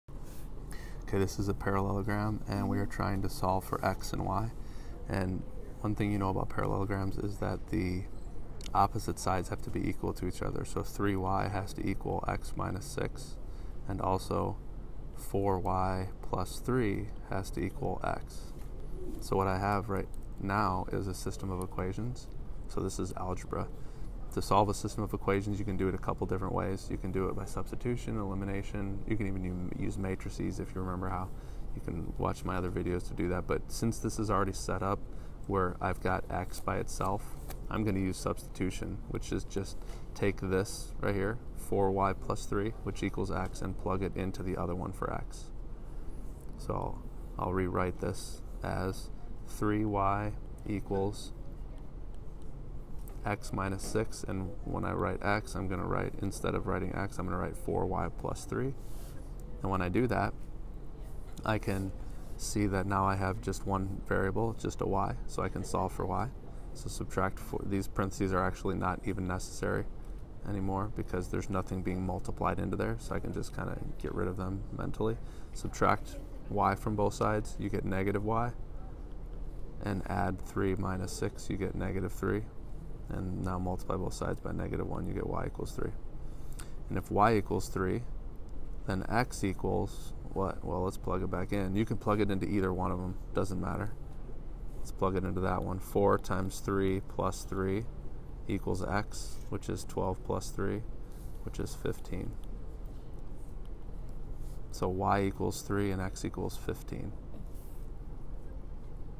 Solving for the variables in the parallelogram
Solve for x and y in a parallelogram, using properties of parallelograms and systems of equations.